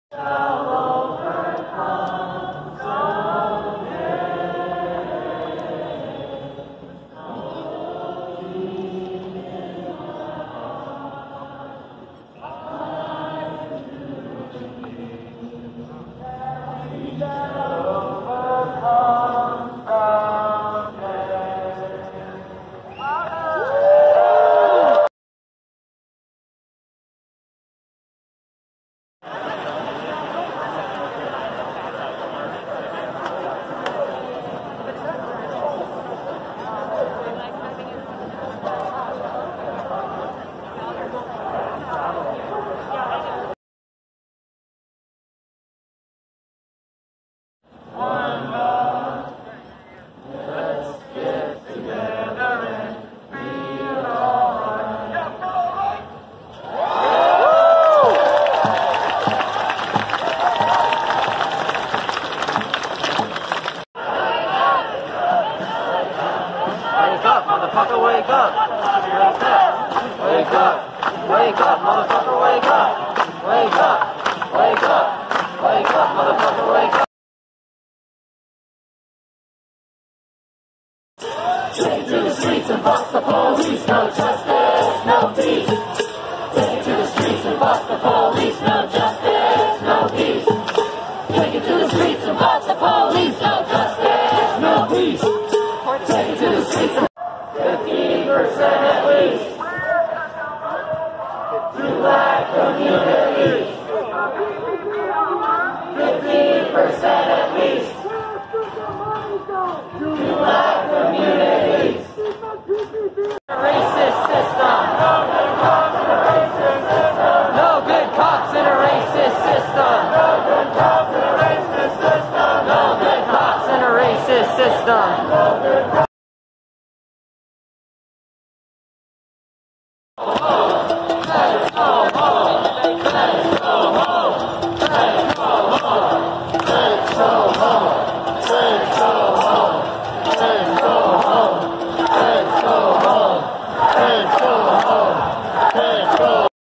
PDX Protest Downtown July 20 Part 1
Part One of a peaceful protest march around the JC and to the murals by the Applestore in Downtown Portland, Oregon.